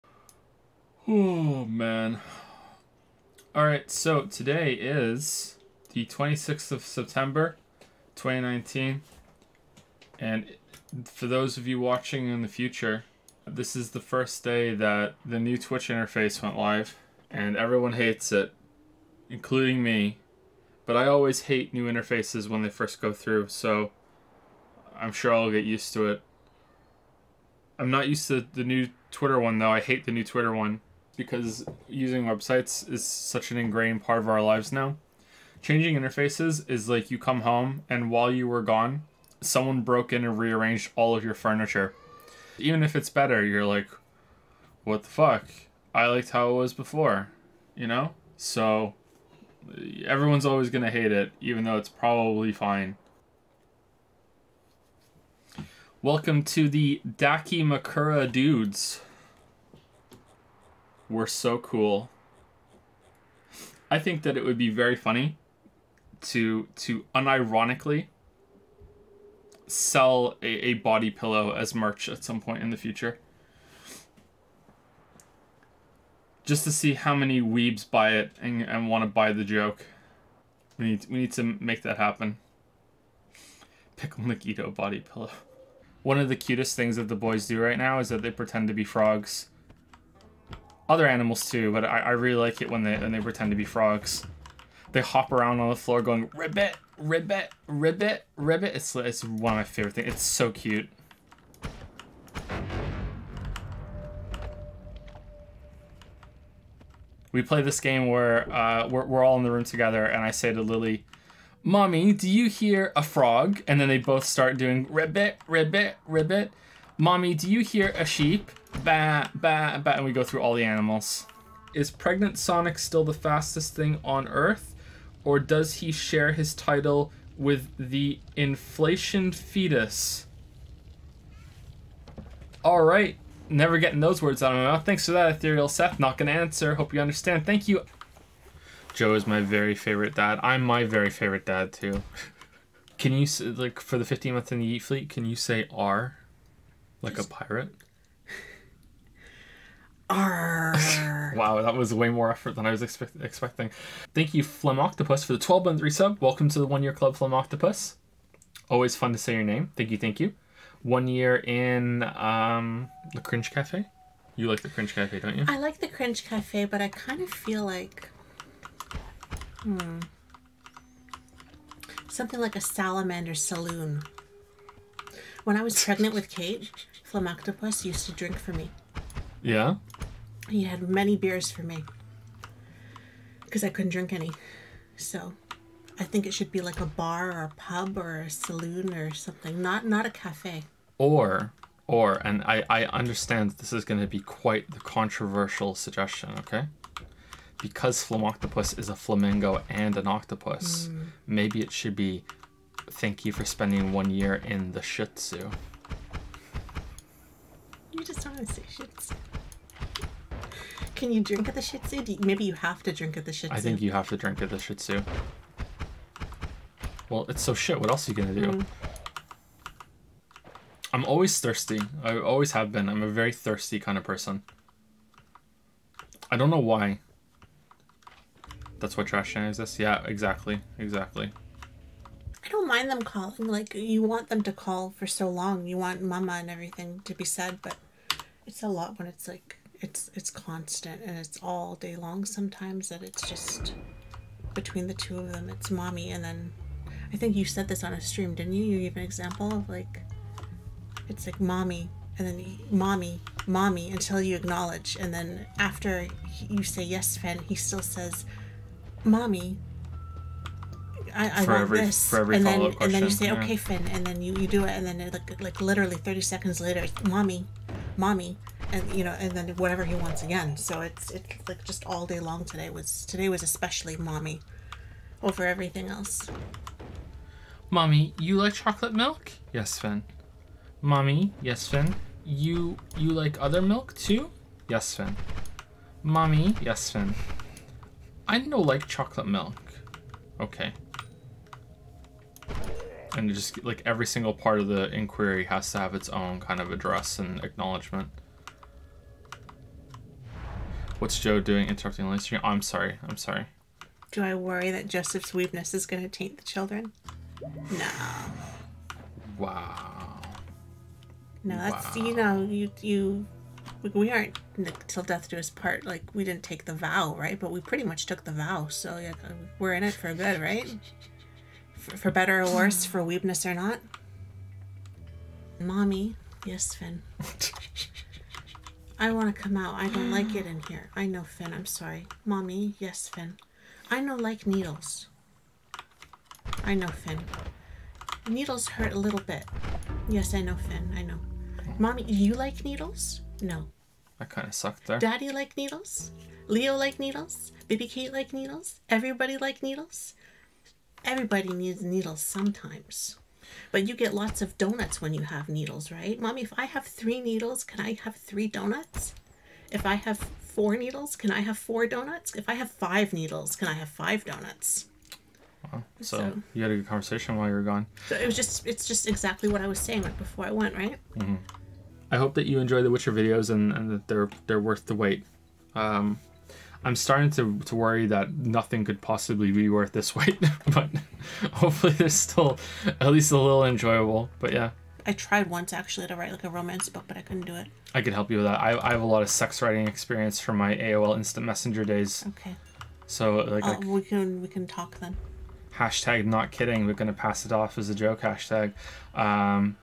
Ribbit, Ribbit, Ribbit!! | Joseph Banterson
Joe and Lili share some stories while blasting away at pixels.
Original stream date: 2019/09/26, 2019/10/26

0:00 - New Interface
0:55 - Body Pillows
1:25 - The Frog Boys
2:01 - Cursed Question
2:18 - Joe, Lili, and Chat
3:52 - Mommy
6:12 - Quips
baa baa baa